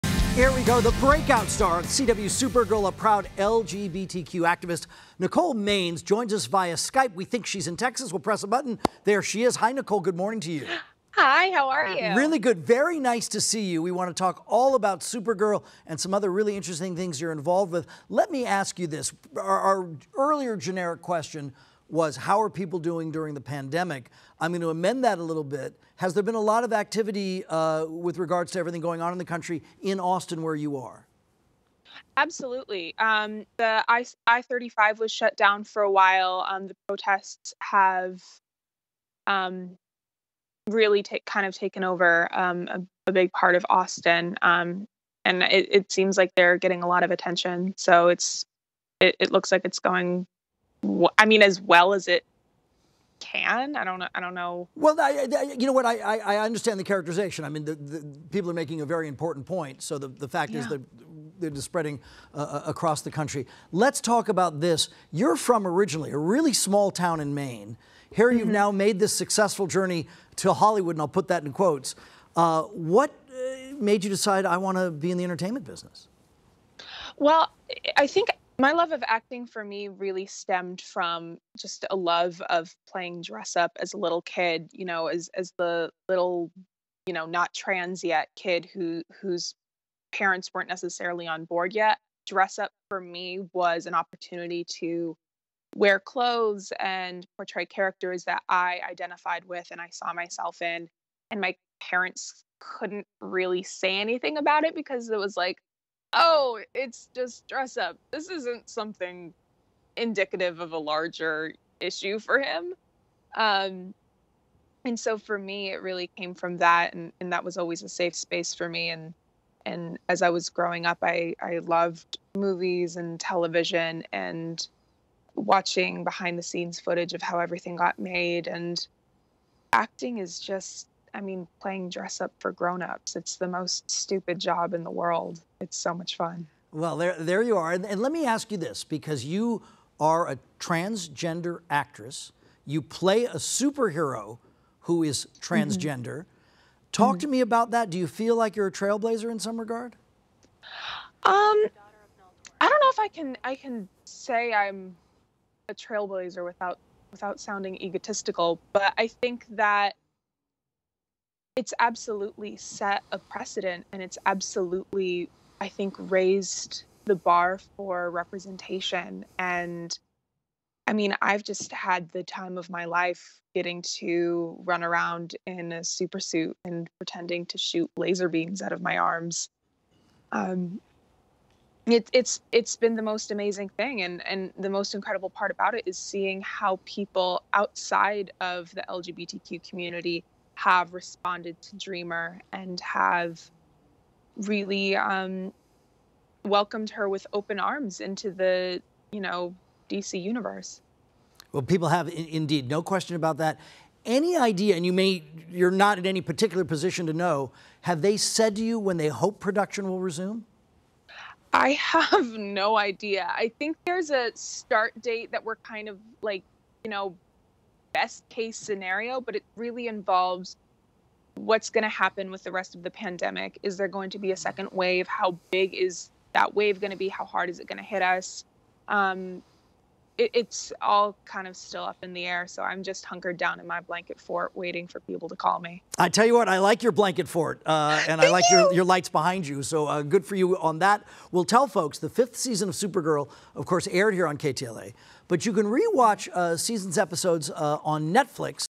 Supergirl's Nicole Maines on playing TV's first Transgender Superhero on The CW
You can catch up on episodes of 'Supergirl' on Netflix.
This segment aired on the KTLA Morning News June 3, 2020.